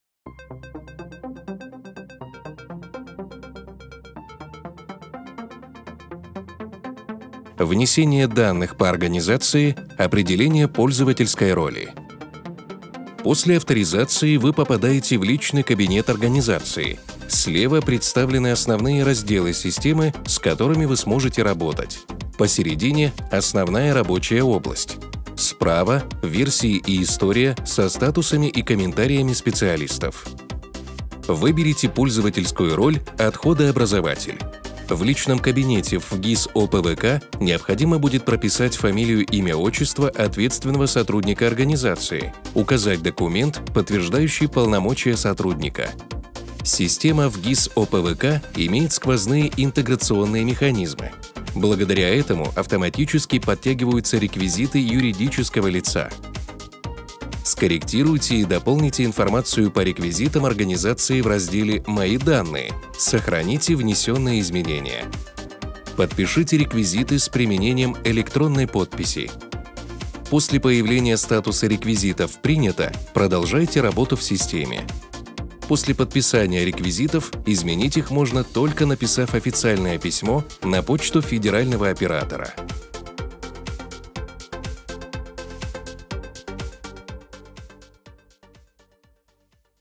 Работа во ФГИС ОПВК – Внесение данных по организации, определение пользовательской роли
Работа в федеральной государственной системе учета и контроля за обращением с отходами I и II классов опасности (ФГИС ОПВК).
Обучающая видеоинструкция о работе в федеральной государственной информационной системе учета и контроля за обращением с отходами I и II классов опасности (ФГИС ОПВК) в рамках реализации федерального проекта «Инфраструктура для обращения с отходами I-II классов» национального проекта «Экология».